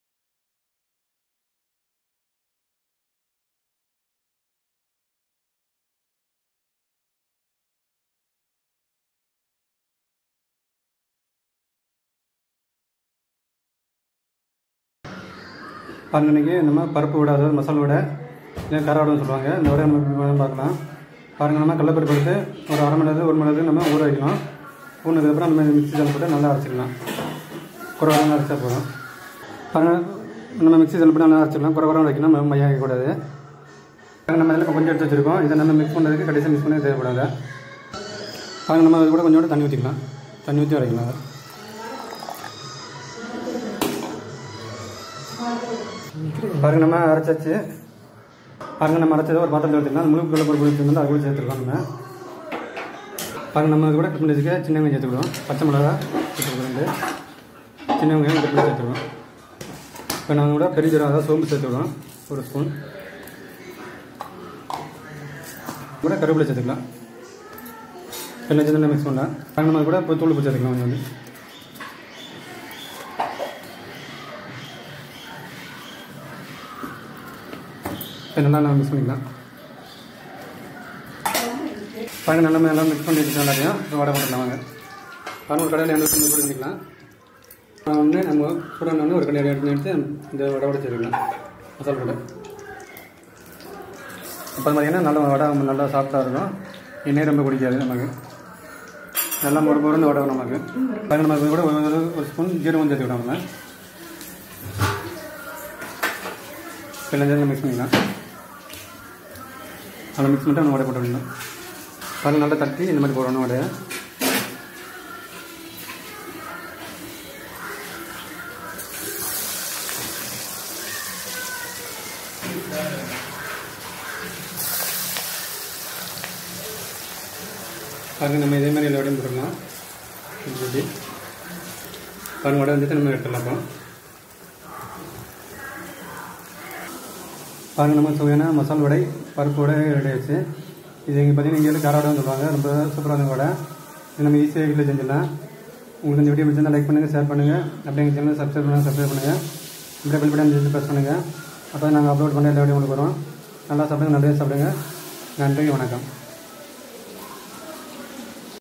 Masal Vadai | மசால் வடை | Paruppu Vadai | பருப்பு வடை | Kara Vadai | கார வடை | Masala Vadai
தேவையான பொருட்கள்
கடலை பருப்பு
சின்ன வெங்காயம்
பச்சை மிளகாய்
பெருஞ்சீரகம்
சீரகம்
கறிவேப்பிலை